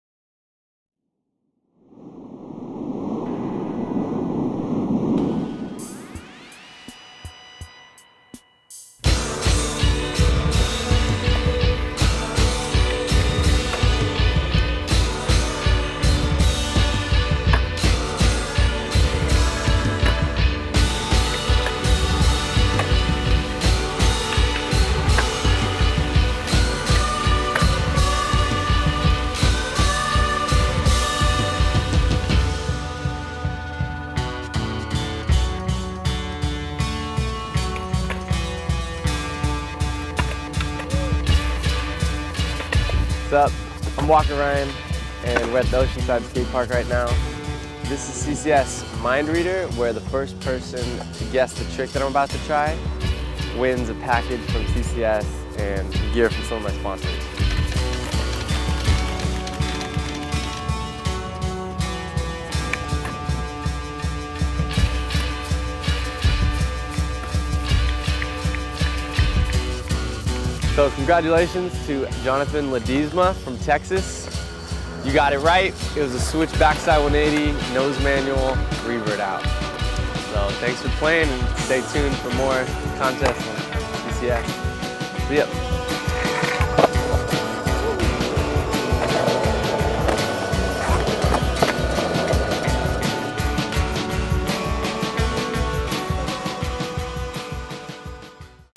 Mind Reader With Walker Ryan
Welcome back Mind Readers! Mind Reader is the contest where you get to test your ESP skills. We ask you to read the mind of a pro for a chance at winning product.

Yesterday we were at Bishop Skate Park in Oceanside California with Walker Ryan and tweeted this photo with the following clues:

"(Hint #1): He's rolling up switch. (Hint #2): No flips are involved. (Hint #3): He rolls out switch."
The first person who correctly read Walker's mind and tweeted the name of the trick to @CCS_Skate_Shop won Mind Reader and a box from C1CRA!  Let's see who that winner was!  Remember to follow @CCS_Skate_Shop on Twitter to play the MIND READER next time.